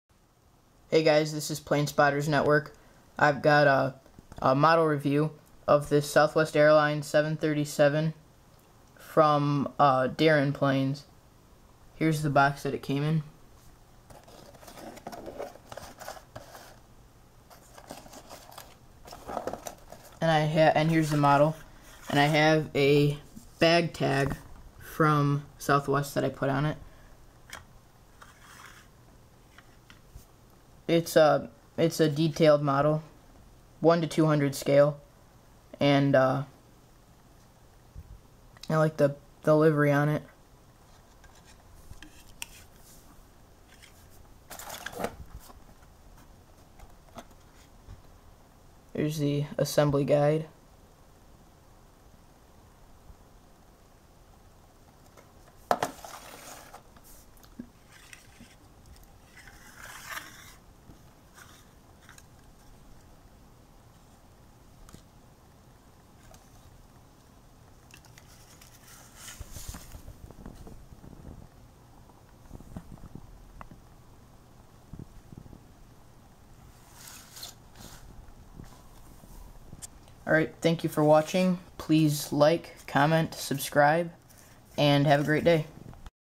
[MODEL REVIEW] Southwest Airlines B737-700 1/200 Scale
SWA B737-700 from Premier Planes! Great snap-fit 1:200 scale plastic model.

Next video: Skymarks Delta A330 model review 1/200